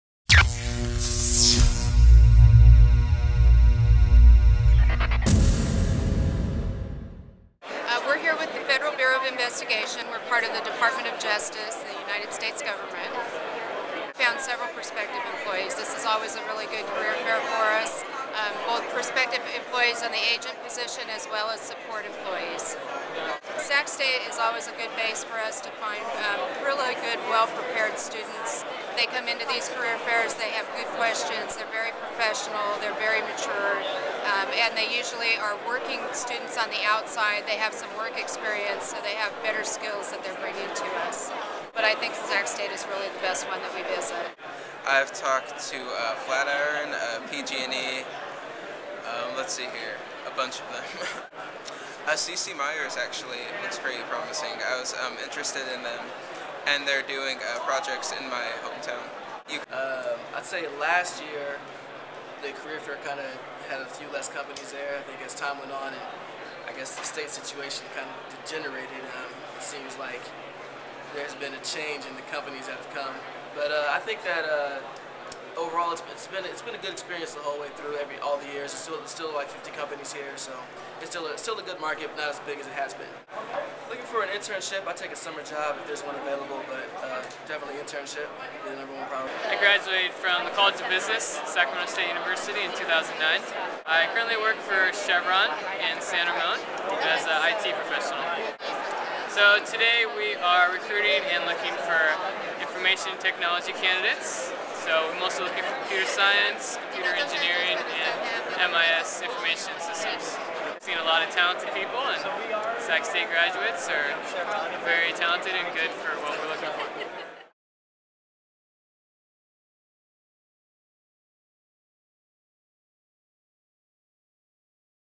Engineering Department Career Day
About 50 companies visited Sacramento State today in the University Union Ballroom during the engineering departments 2010 Career Day.